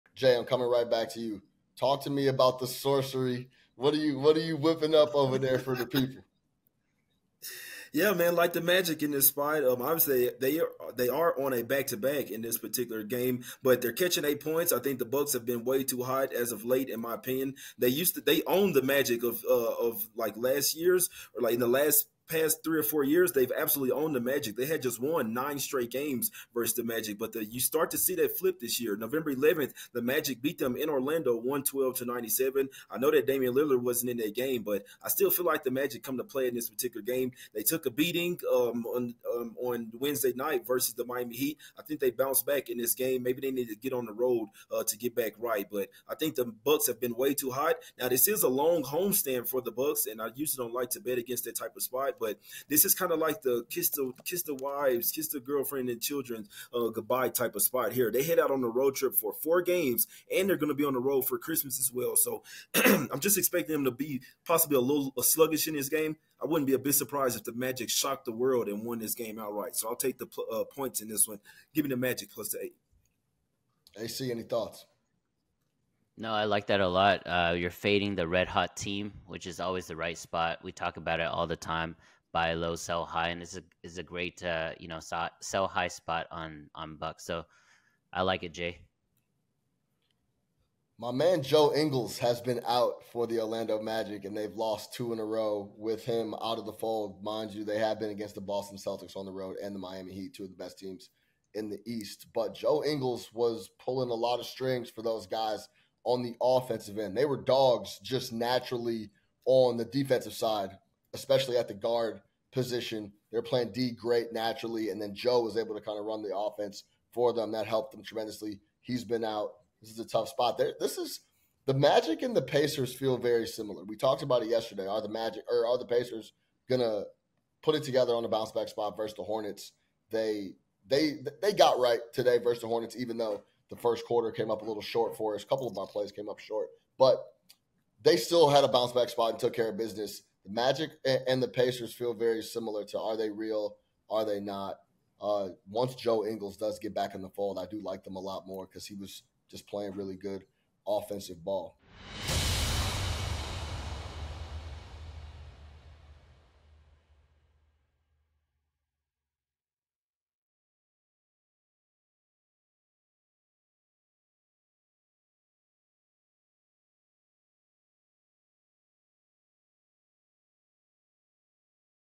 Orlando Magic vs Milwaukee Bucks NBA Best Bets | NBA Picks & Predictions | Buckets
Action Network NBA experts Jay Money and Albert Nguyen join Sean Little to discuss the matchup of the Orlando Magic vs Milwaukee Bucks and give out their NBA best bets.